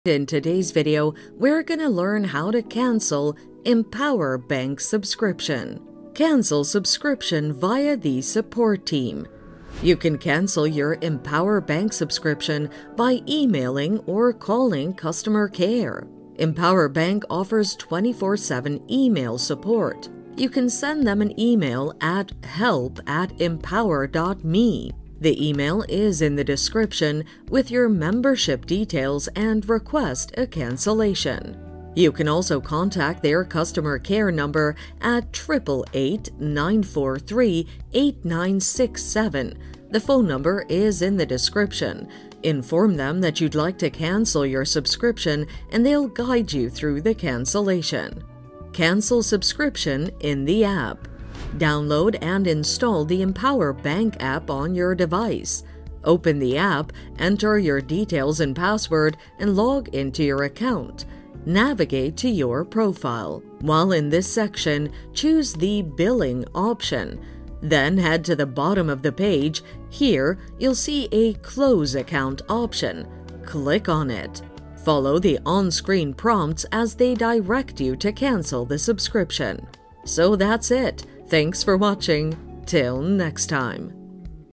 How To Unsubscribe From Empower (How To Cancel Empower Bank Subscription)
How To Unsubscribe From Empower (How To Cancel Empower Bank Subscription). In this video tutorial I will show how to unsubscribe from your Empower Bank subscription.

Cancel subscription Via the Support team.
You can cancel your Empower bank subscription by emailing or calling customer care. Empower Bank offers 24/7 email support.
● You can send them an email at (The email is in the description.) with your membership details and request for a cancellation.
● You can also contact their customer care number at (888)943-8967 (The phone number is in the description) to inform them that you’d like to cancel your subscription, and they will guide you through the cancellation.

Cancel subscription In the App.
● Download and install the Empower Bank App on your device.
● Open the app, enter your details and password, and log in to your account.
● Navigate to your profile.
● While in this section, choose the “Billing” option.
● Then head to the bottom of the page; here, you will see a “Close account” option; click on it.
● Follow the on-screen prompts as they direct you to cancel the subscription.